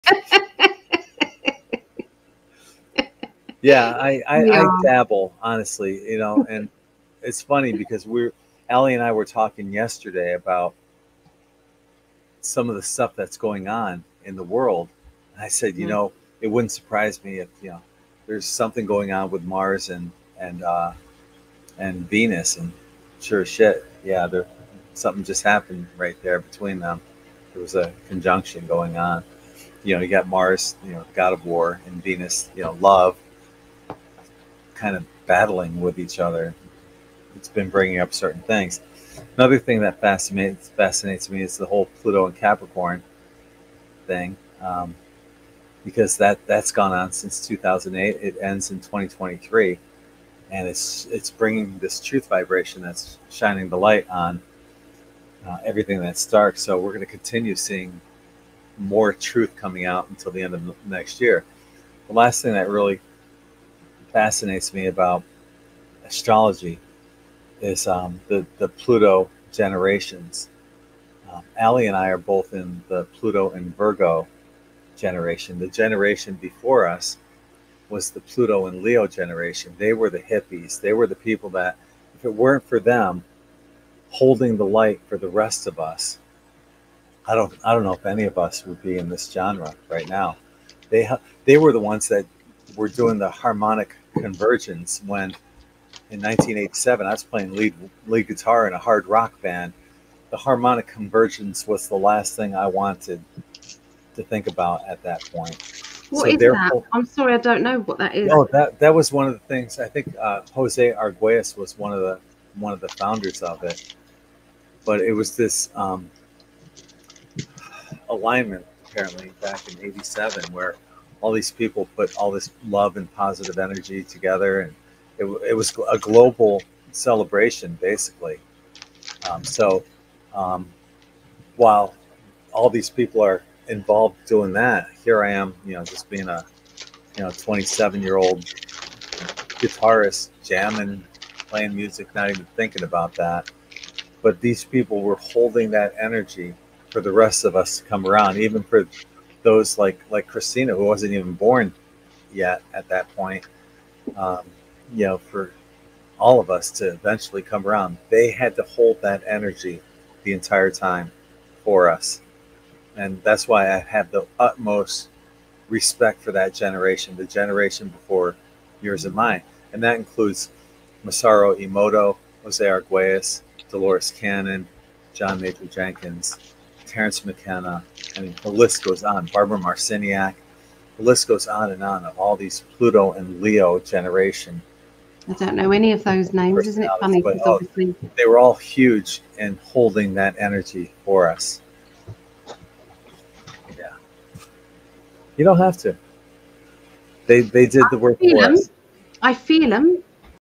Pluto in Leo Generation - Spiritualist Should All Be Thankful
Each generation has its own energy signature. The Pluto in Leo generation was between June 14, 1939 through August 19th, 1957. Even siblings with only a few years difference can be in two different generations.  Both of my sisters were born between 1956-57 and fall into the Pluto in Leo Generation.  I was born a few years later and I'm in the Pluto in Virgo generation, which has its own energy signation and generational challenges.
Check out our Mystic's Portal Gift Shop with over 6,000 products!